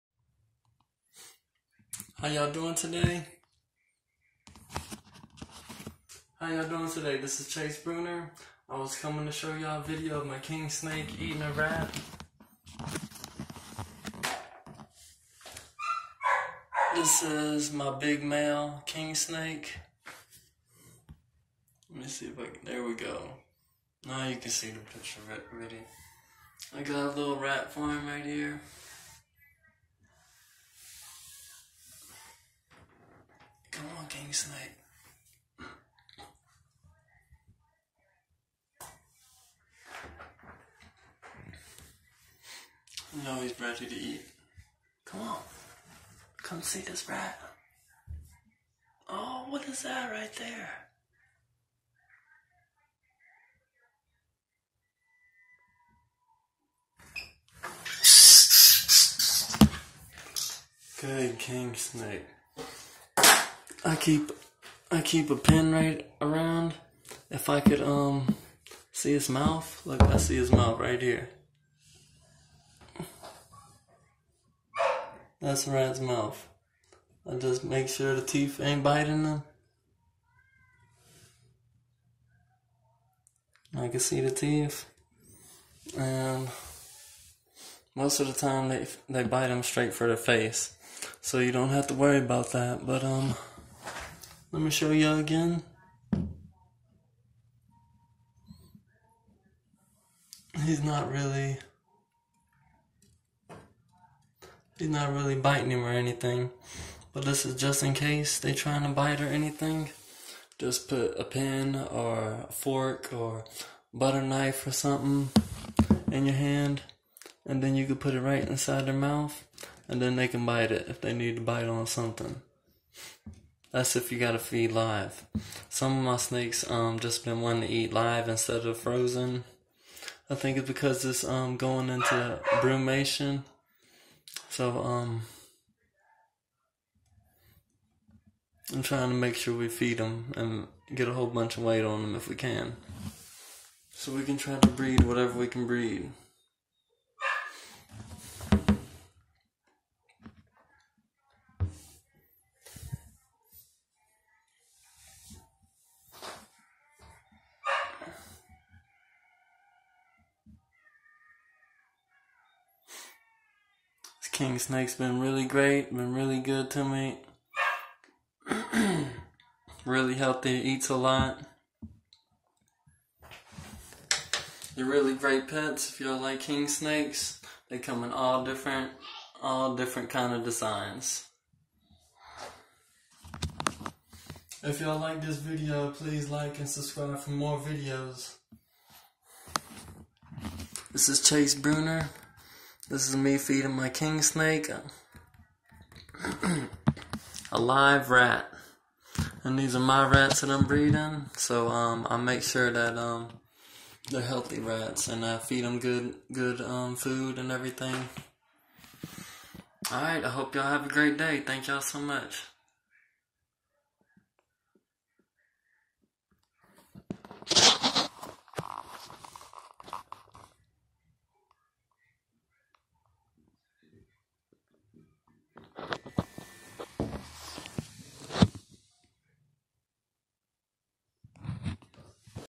FEEDING THE KING 👑 SNAKE 🐍 A RAT 🐀 !!! | CHASE BRUNNER
What's Up Everyone Hope You All Are Having A Wonderful Day So Far Today !!!
I Been Taking Care Of The Animals Most Of My Day !!

Sharing With You A King Snake Feeding Video Eating A Live Rat !!!